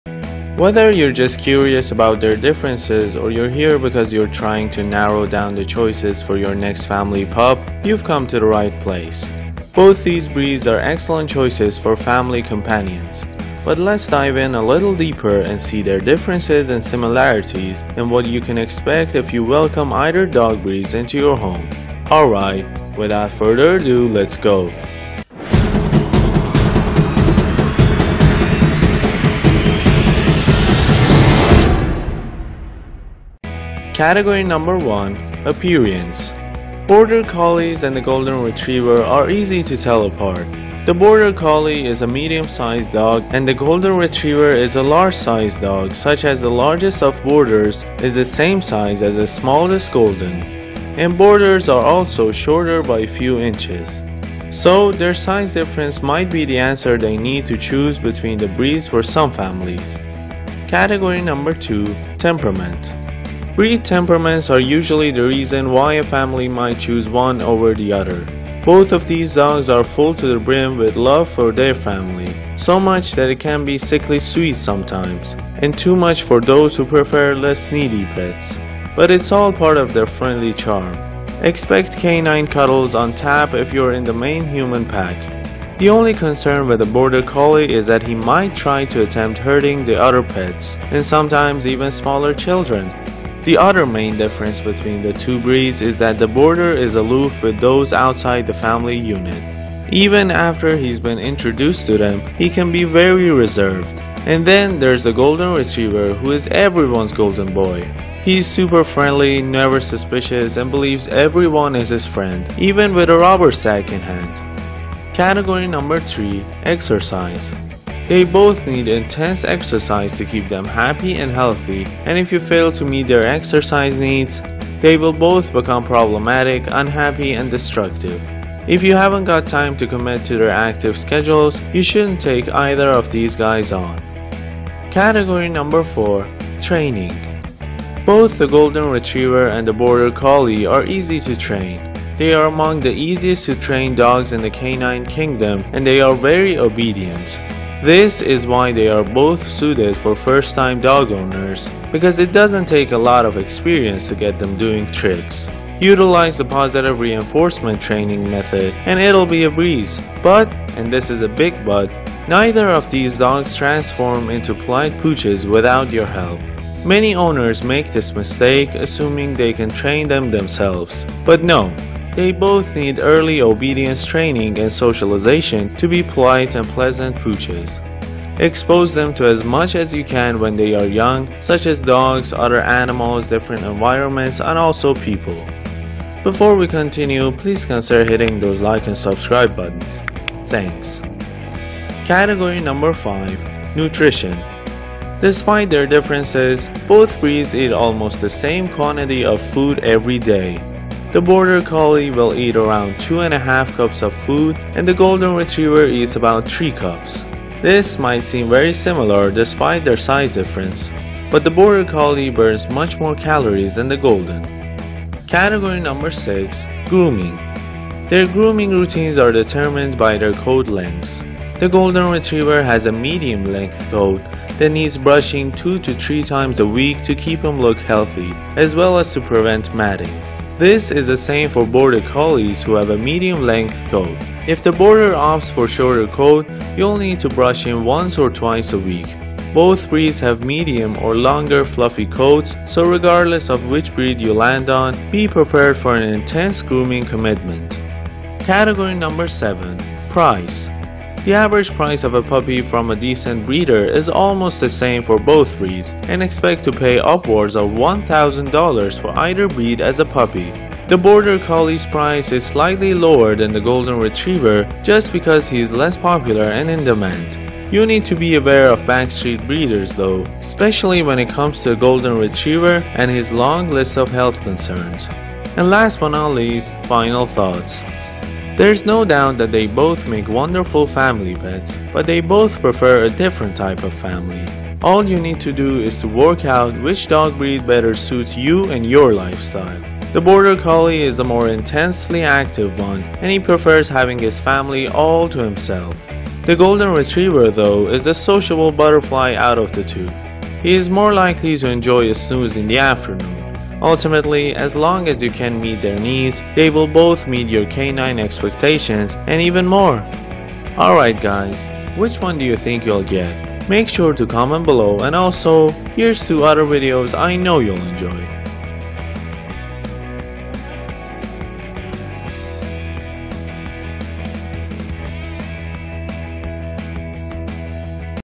Golden Retriever vs. Border Collie - Which Should You Get?
In this video we'll go the Differences & Similarities of the BORDER COLLIE & the GOLDEN RETRIEVER and which one should you get! Let's get started!
Golden retriever, border collie , dogs , dog , pet , pets , buy , family , families , cheap , budget , low-budget , low-cost , what to buy , buyguide